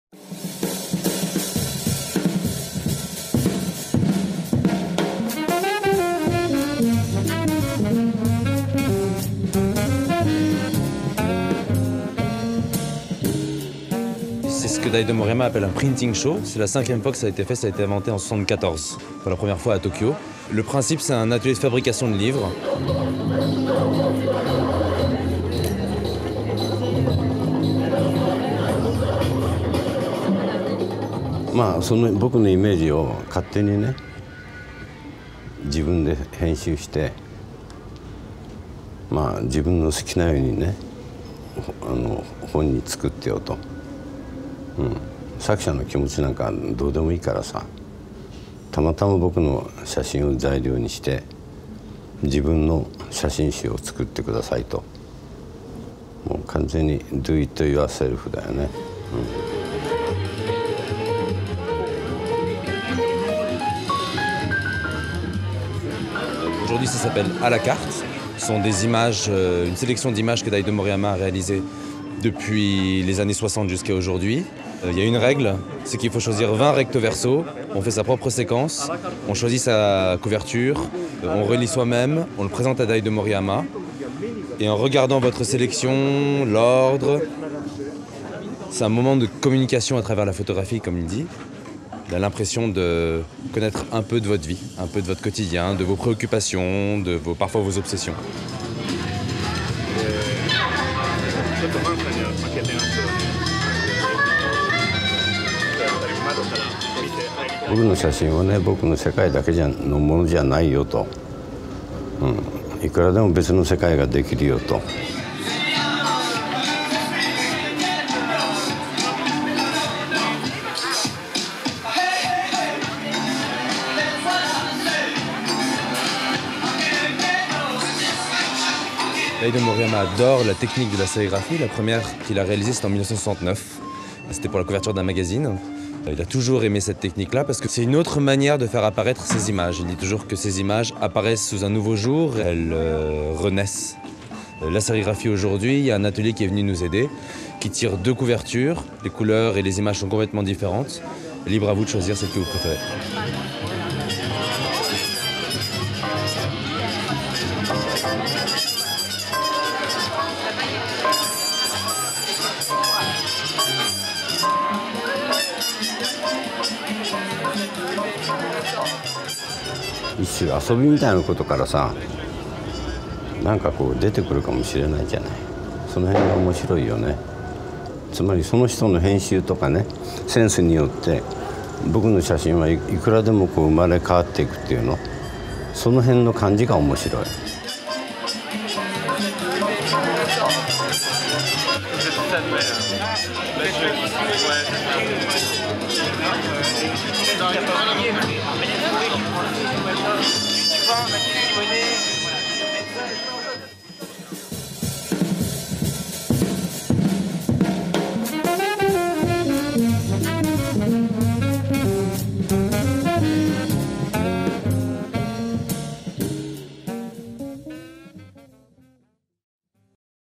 Daido Moriyama, À la carte - Printing show | Les Soirées Nomades - février 2016
Daido Moriyama, À la carte
[Printing show]

En 1974, Daido Moriyama réalise à Tokyo son premier atelier participatif de fabrication de livre qu'il intitule printing show. Dans le cadre de l’exposition Daido Tokyo à la Fondation Cartier, les Soirées Nomades organisent la cinquième édition sous le titre À la carte.

Performance éphémère, le printing show offre au visiteur l’expérience de réaliser son propre livre : chacun est invité à sélectionner 40 images parmi les 60 proposées par Daido Moriyama et en définit l’ordre afin de créer son exemplaire unique. Sur place, les pages sont imprimées et assemblées avec une couverture sérigraphiée ; le livre est ensuite signé par l’artiste dans l’effervescence d’un atelier animé par le bruit des photocopieurs en marche.

« Lorsque je signe les livres, je regarde la première page et je me dis : « Ah. Cette personne a choisi cette image ! » J’ai l’impression de connaître ses goûts et son caractère sans pourtant ne rien savoir de son quotidien ou de son travail. […] J’aime beaucoup cette idée. Je pense qu’il existe, grâce à la photographie, un moment de communication avec celui ou celle qui se tient devant moi à ce moment là. » Daido Moriyama

Daido Moriyama, À la carte est un événement co-produit par la Fondation Cartier pour l’art contemporain, la Daido Moriyama Photo Foundation et les galeries Akio Nagasawa (Tokyo) et Jean-Kenta Gauthier (Paris).

Avec la participation de Franck Stofer (DJ set)